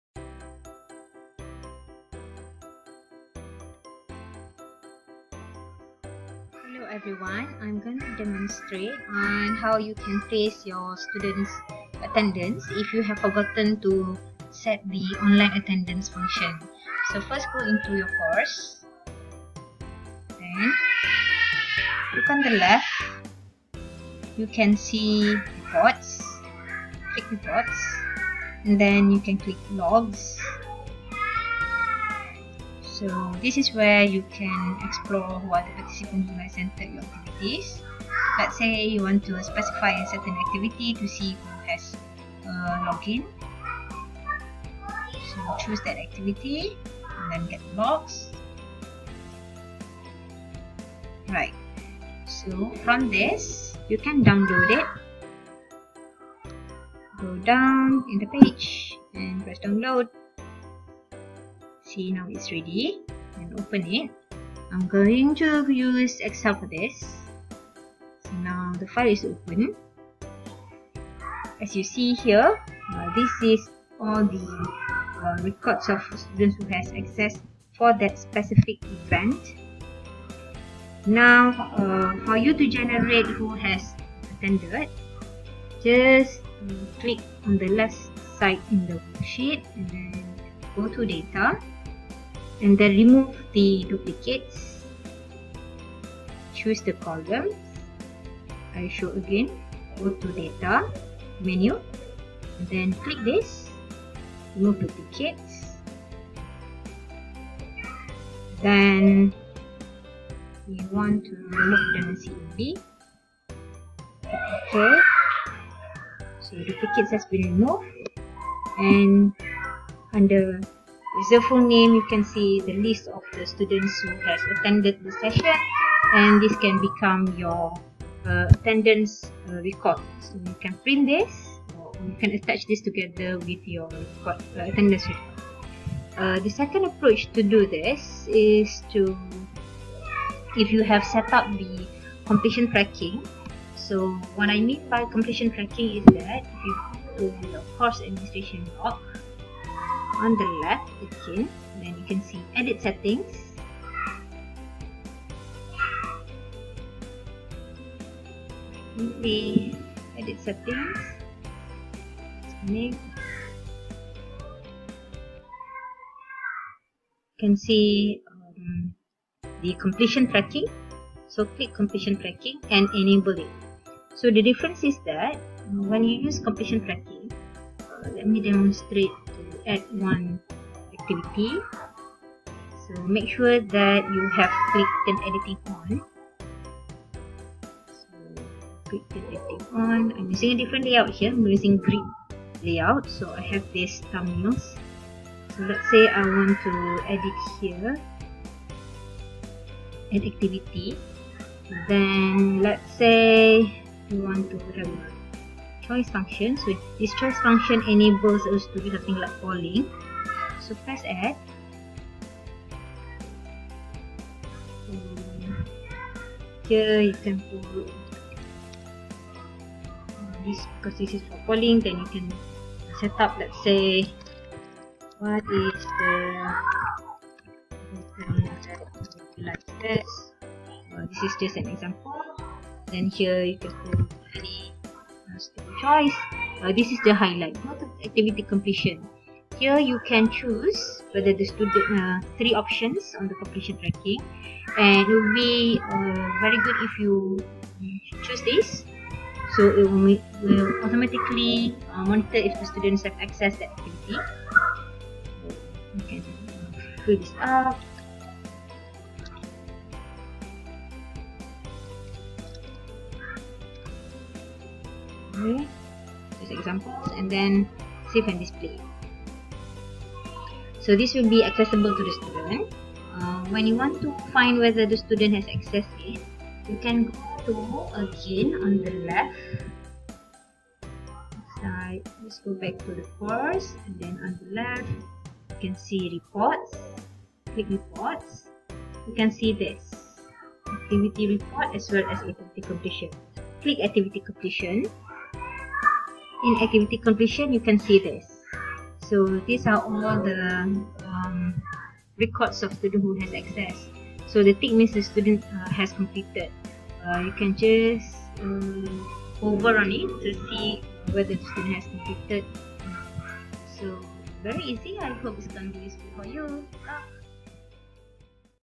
PutraBLASTv3 Tutorial Trace Attendance
PutraBLAST is the learning management system at Universiti Putra Malaysia and it is based on Moodle. This tutorial provides steps to trace online attendance without using the Online Attendance function. At least two methods can be utilised; by tracing in the log and checking action completion.